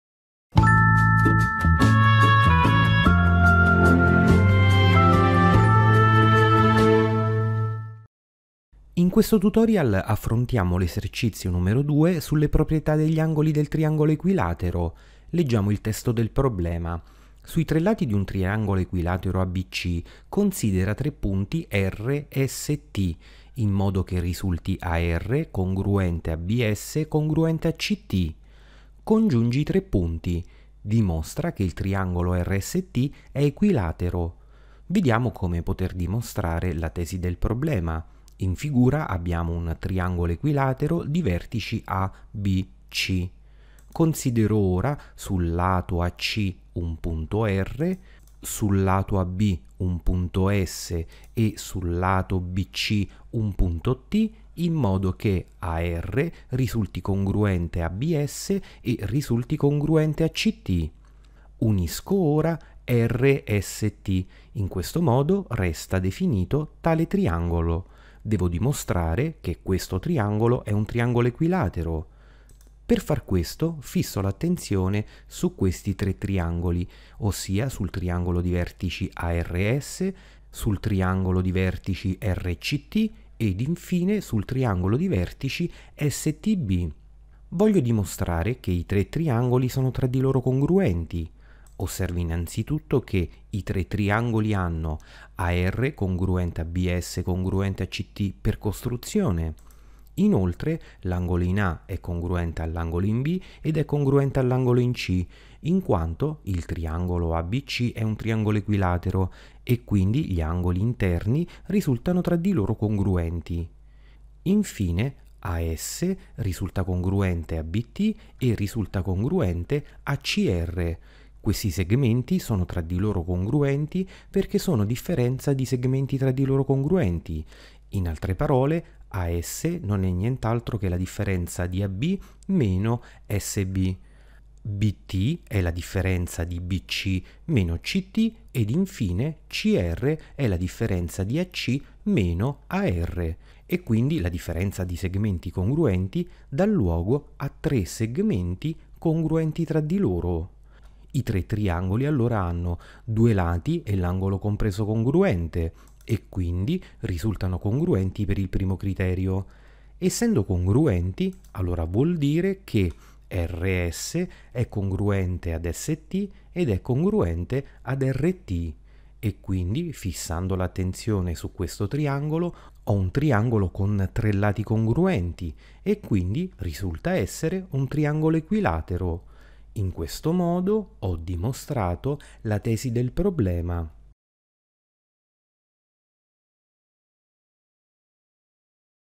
Esercizio n.2 - le proprietà degli angoli di un triangolo equilatero
Vediamo un problema di geometria risolubile con la proprietà degli angoli di un triangolo equilatero.